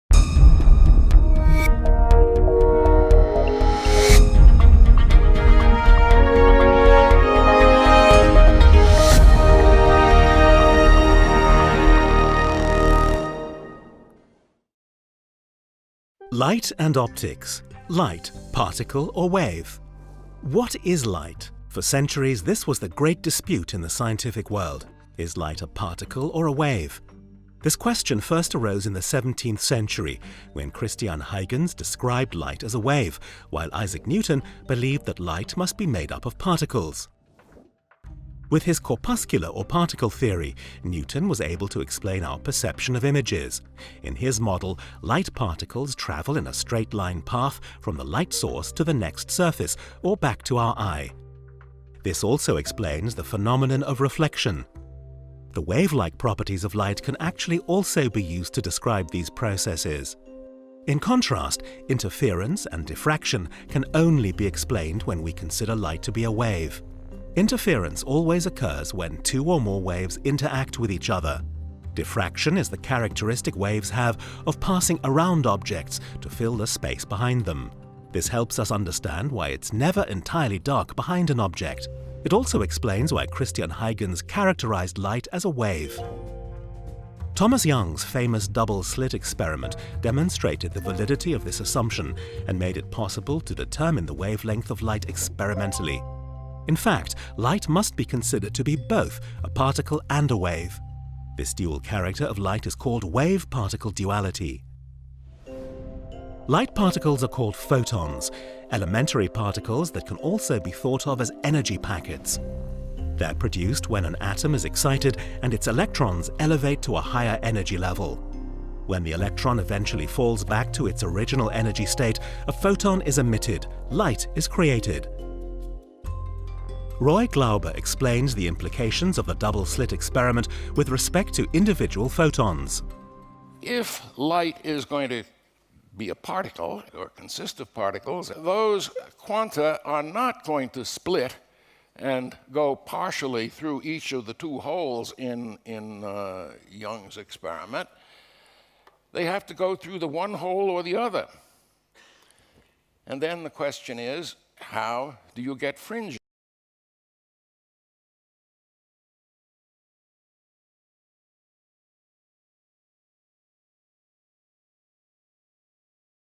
Light and Optics III: Light - Particle or Wave?
How can one describe light? Is light made up of particles or should one rather describe light as wave? How does one best explain the various phenomena in connection with light? The answers to these questions are given in part three of our five-part Mini Lecture series on “Light and Optics”.